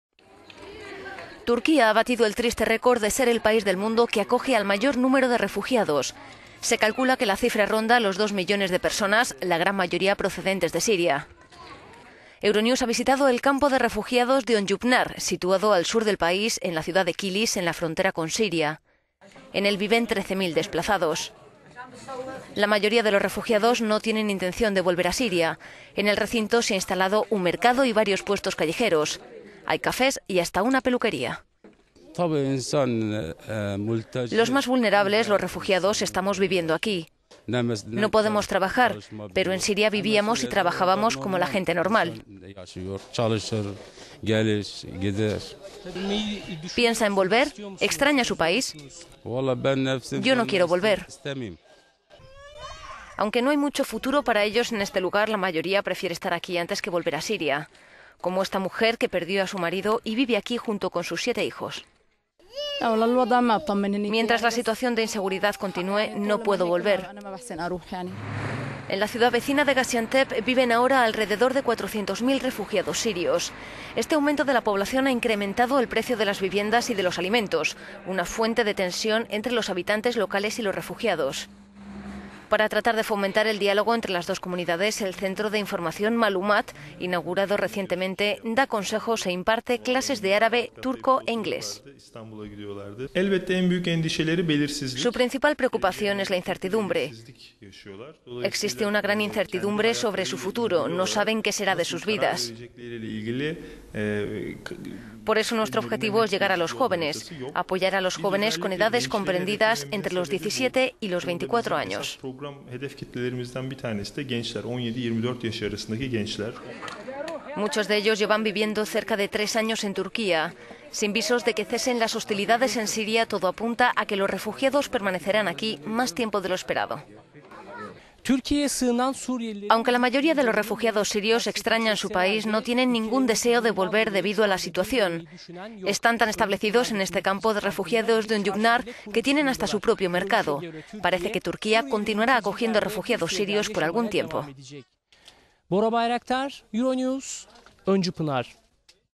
La "pequeña Siria " de Turquía
Turquía ha batido el triste récord de ser el país que acoge al mayor número de refugiados. Se calcula que la cifra ronda los dos millones, la gran mayoría procedentes de Siria.

Euronews ha visitado el campo de refugiados de Öncüpınar, situado al sur del país, en la ciudad de Kilis, en la frontera con Siria. En él viven 13.000 desplazados. Se le conoce como la "pequeña Siria".

La mayoría de refugiados no tienen intención de volver a Siria. En el recinto se ha instalado un mercado y varios puest…